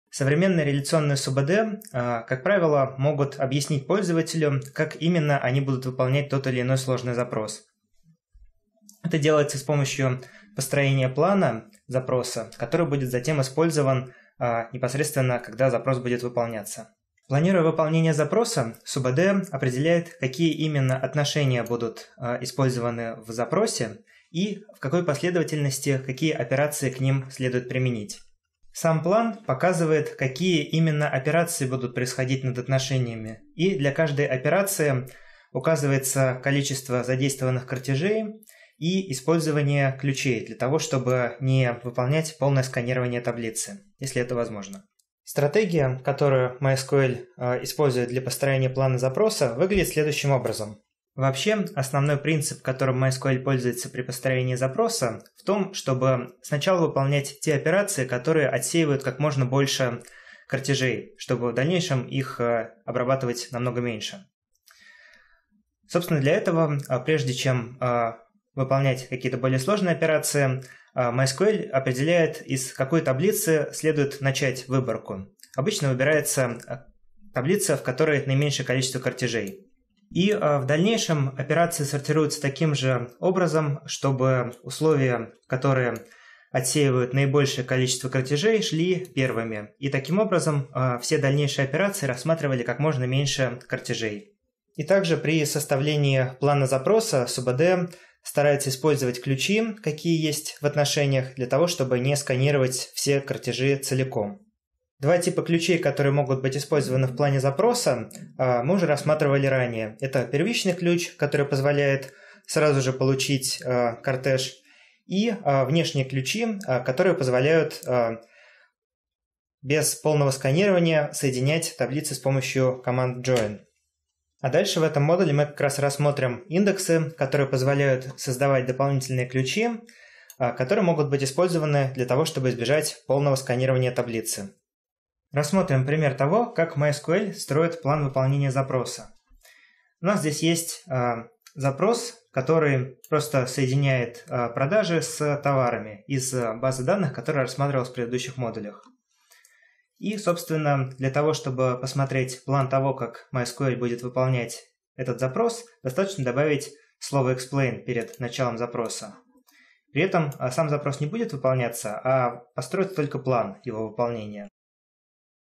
Введение в базы данных - 77 урок. Сложность выполнения запросов. EXPLAIN
Автор: Александр Мяснов, Иван Савин

Задание для закрепления:

Для чего используется план выполнения запроса, выстраиваемый командой EXPLAIN?

Выберите все подходящие ответы из списка:

* Определение количества кортежей, которые будут обработаны во время выполнения запроса

* Для определения точного времени выполнения запроса

* Проверка используемых индексов

* Проверка порядка выполнения операций в запросе для последующей оптимизации

* Переписывание запроса более оптимальным способом